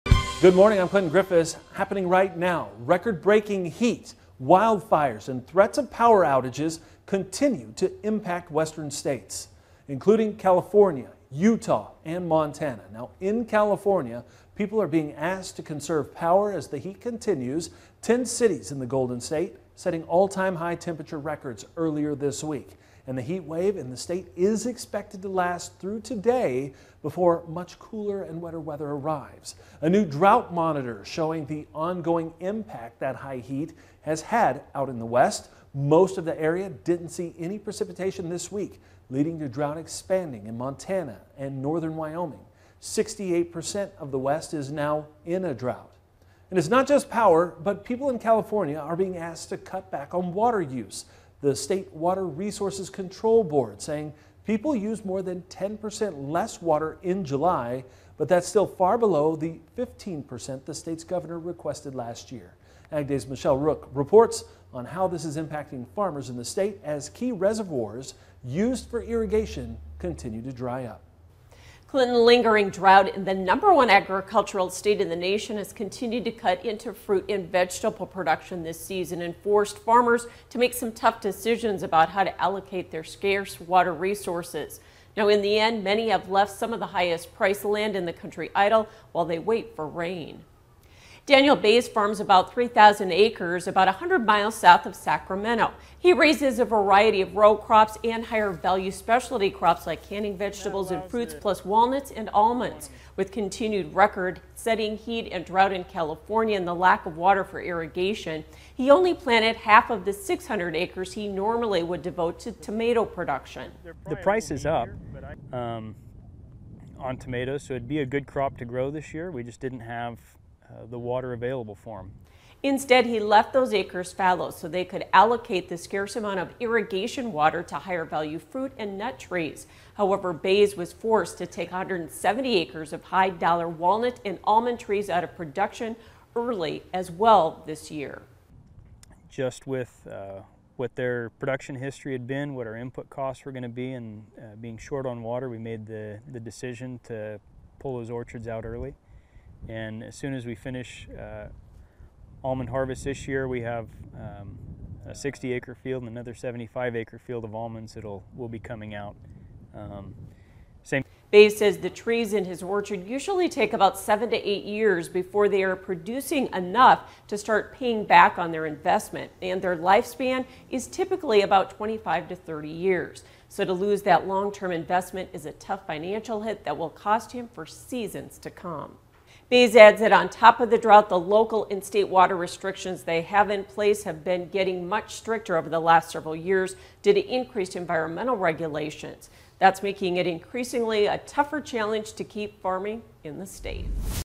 California's Extreme Heat Waves Impact on Farmers
Record breaking heat, wildfires and threats of power outages continue to threaten western states. Farmers are currently dealing with a lack of irrigation.